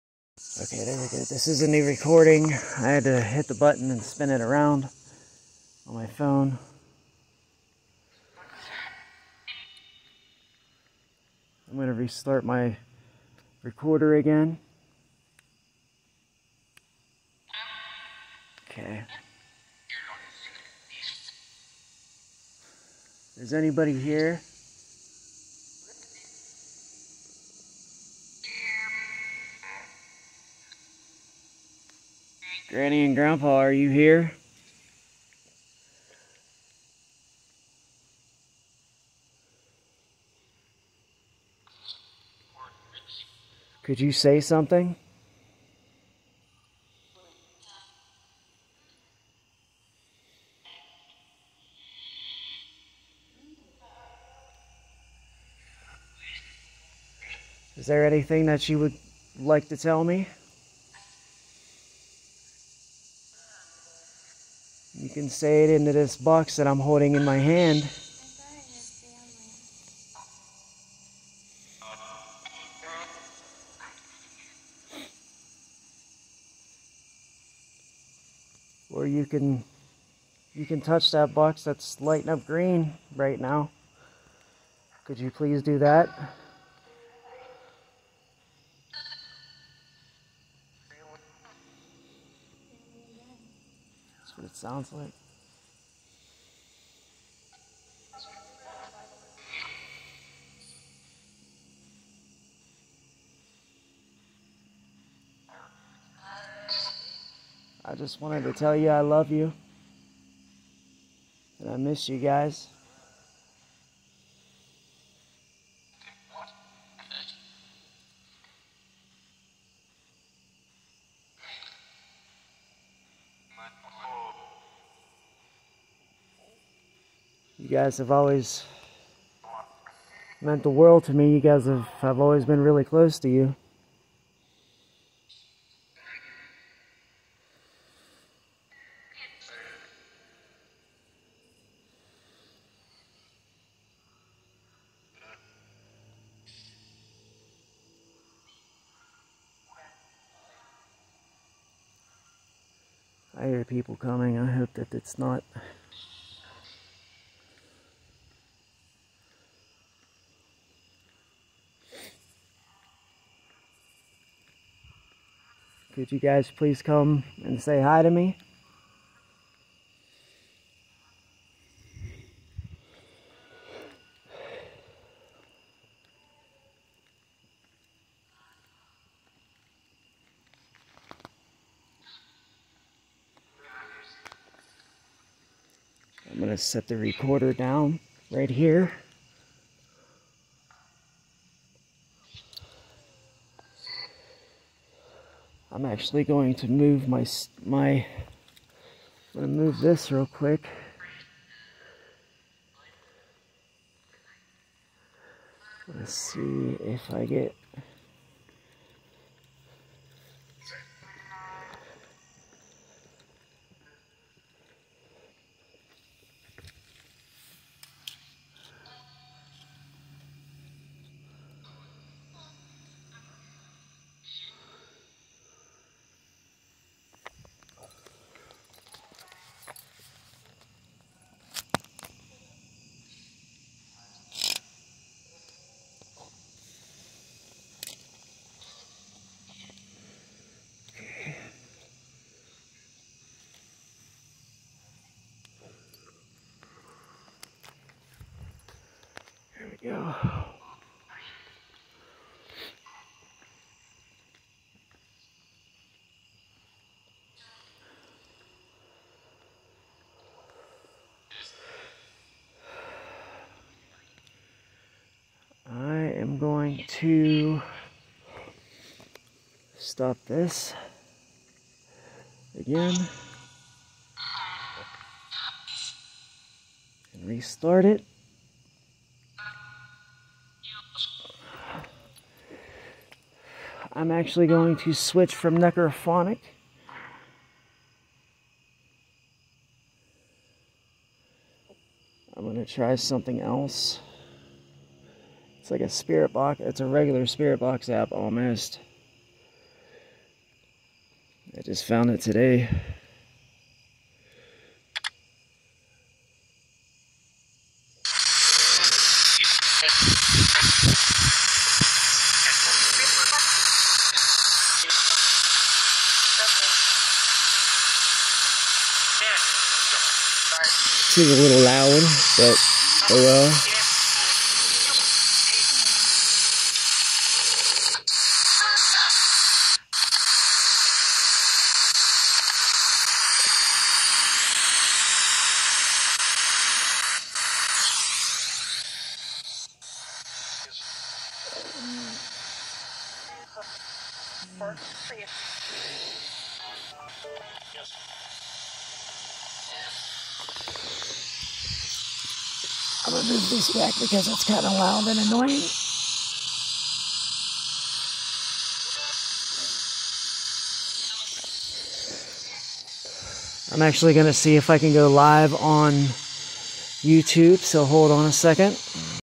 9/3/22- Lafayette cemetery investigation #2
Part 2 of 3 of investigating at the mausoleum where my grandma is laid the rest. If you see or hear anything let me know.